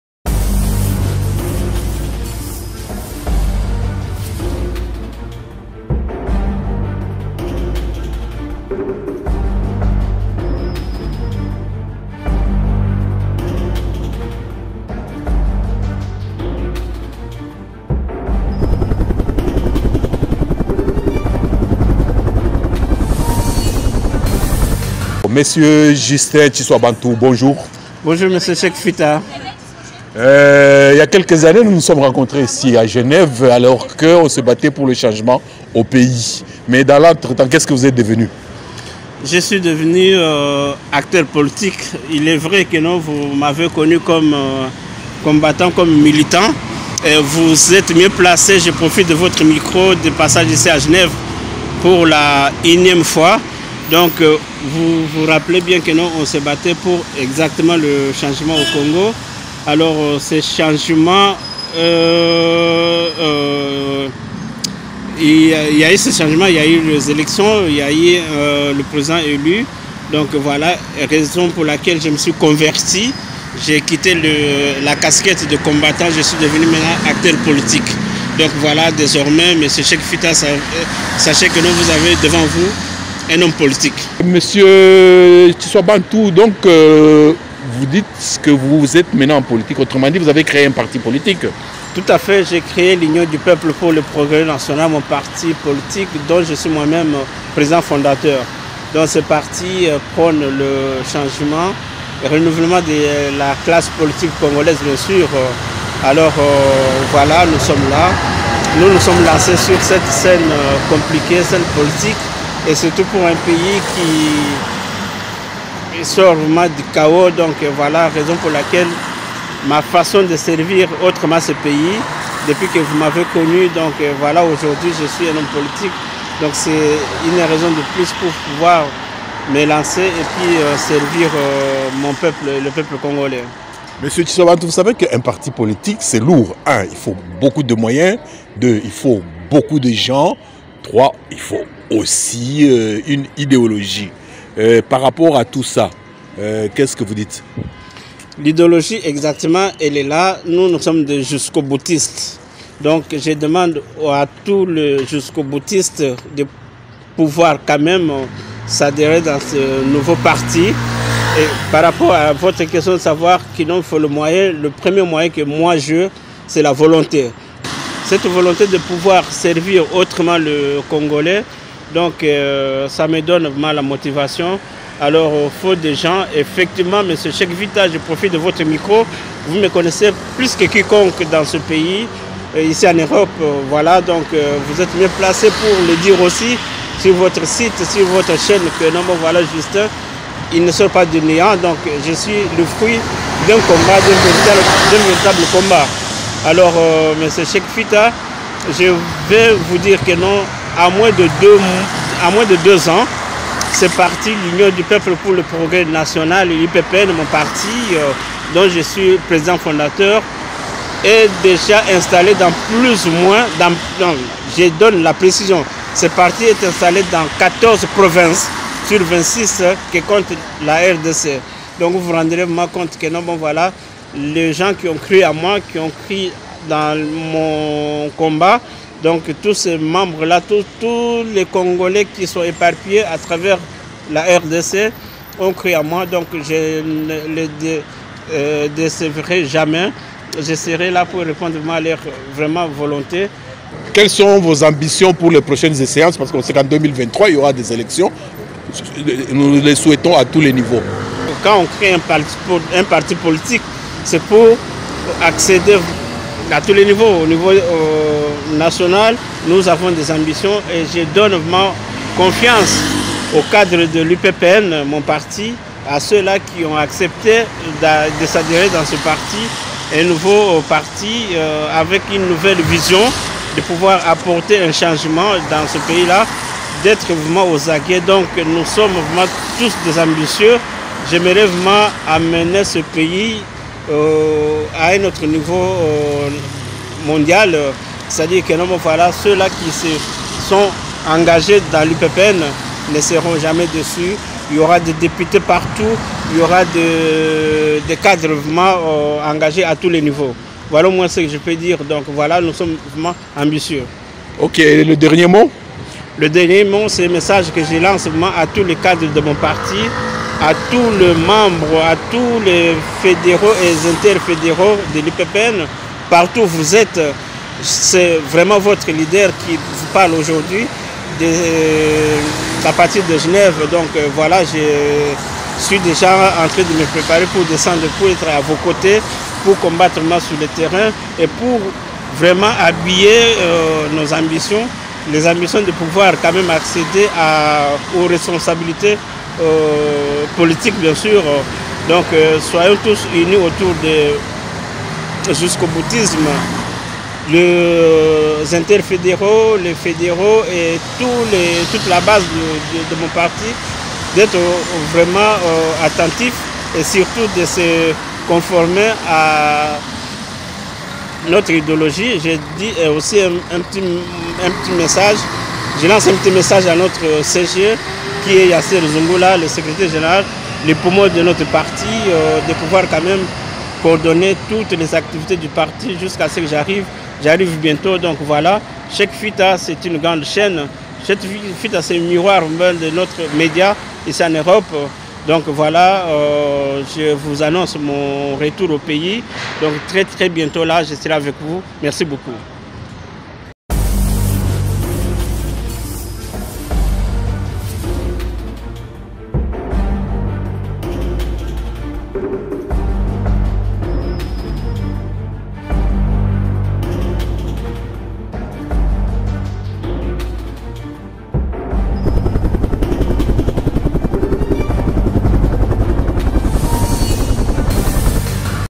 JUSTIN TSHISWABANTU D'ACTIVISTE A PRESIDENT DE PARTI
Bruxelles, le 16 octobre 2021
Par Cheik FITA
Profitant de notre séjour à Genève, lors dans le cadre de  la soixante-douzième session du Comité Exécutif du Programme du Haut-commissariat  des Nations Unies pour les Réfugiés (HCR), l’Info en Ligne des Congolais de Belgique a donné la parole à Mr Justin Tshiswabantu.
Durant des années, Justin Tshiswabantu a été un grand activiste-combattant à Genève.
Il était de toutes les actions, il voyageait même vers Paris et Bruxelles en vue du changement politique en RD Congo.
En 2021, changement de look. Il n’est plus en treillis militaire mais en costume-cravate.
Pourquoi ? Avec le changement au pays et le renvoi dans l’opposition de Joseph Kabila et consorts, il n’y a plus d’enjeux politiques en Europe. Les enjeux sont désormais au pays. C’est ainsi que Justin Tshiswabantu a décidé d’entrer en politique en créant un parti politique : l’UPPN : Union du Peuple pour le Progrès National.
Dans la vidéo ci-dessous, il nous explique pourquoi cette mutation.